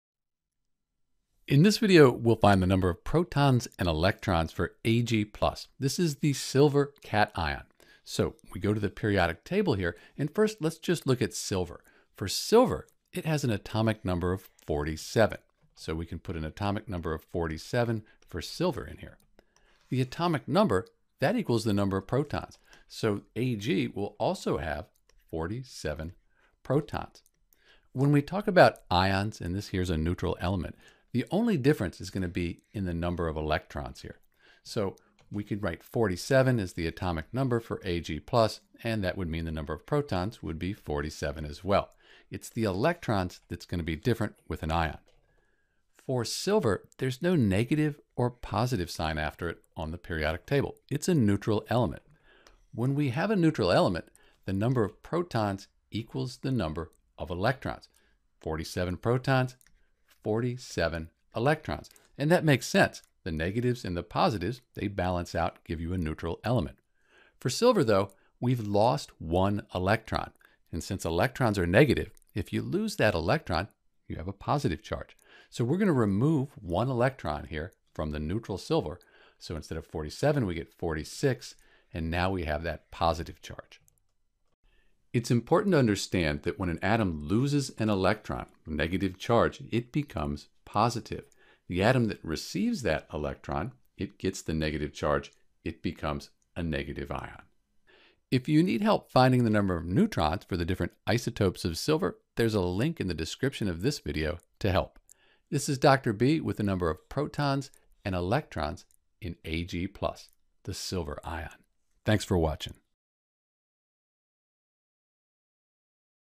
How to find Protons & Electrons for Ag+ (Silver ion)
In this video we’ll use the Periodic table and a few simple rules to find the number of protons and electrons for the Silver ion (Ag+). From the Periodic Table we can find the element symbol, atomic number, and atomic mass.  Using this information we can find the other information for the Ag+ ion.

Note that even though Ag is a transition metal it only has forms the Ag+ ion.

Rules

Atomic Number = Number of Protons

Number of Protons = Number of Electrons         (for a neutral element)

***For ions the only difference is in the electron.***

When at atom loses electrons a positive ion is formed.  For example, with Ag+, the + tells you that one electron was lost.

When at atom gains electrons a negative ion is formed.  For example, with N 3-, the 3-  tells you that three electrons were gained.

With something like Na+  or Cl-. we assume that we have 1+ and 1- even though the number "1" is not written.

Information on the Number of Neutrons and Mass

Mass Number = Number of Protons + Neutrons

The Atomic Mass is usually written below the number.  If you are confused the Atomic Mass will be the larger number – think of it as more massive than the Atomic Number.  Often it also has decimals.

If you are not given the Mass Number, it’s best to round to the Atomic Mass to the nearest whole number.  For example, with Carbon (C) we have 12.01.  We can round this to 12.

Note, that sometimes, for example,  you are given a Mass Number of 1 for Hydrogen. This can happen because Hydrogen atoms usually have 0  neutrons.   But sometimes a H atom will 1 or 2.  These are the three different isotopes for H.   They have the same number of protons but  different numbers of neutrons and therefore a different Mass Number.